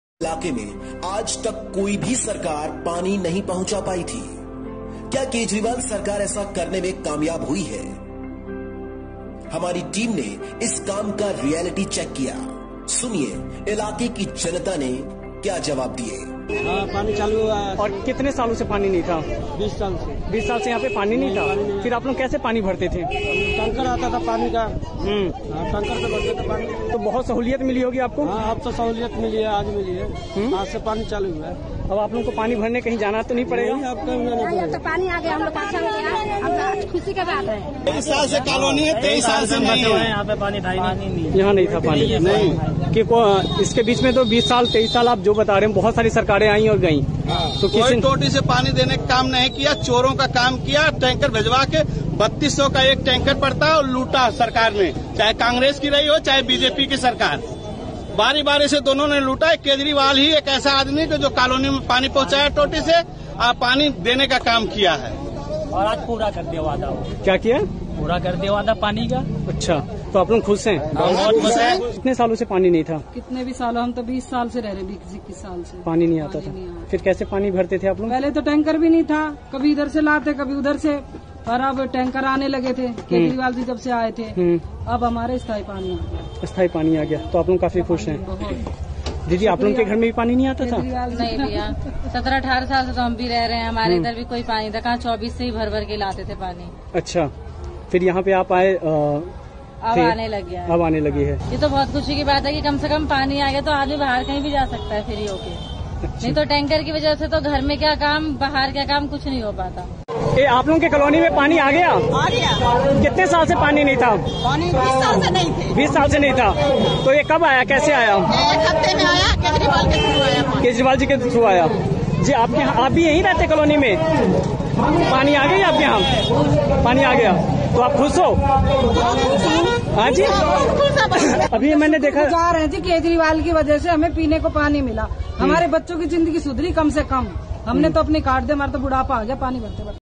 Water supply is being improved through pipeline by DJB under Kejriwal Govt.
जब दिल्ली की उस कॉलोनी में पानी की पाइपलाइन पहुंचती है जिसमें सालों से लोग पानी सप्लाई की दिक्कत झेल रहे थे तो उसके बाद वहां लोग  अरविंद केजरीवाल को दुआएं ही देते हैं।

ये वीडियो खुद भी देखिए और इसे ज्यादा से ज्यादा शेयर कर दूसरों को भी ये दिखाइए - 👇👇👇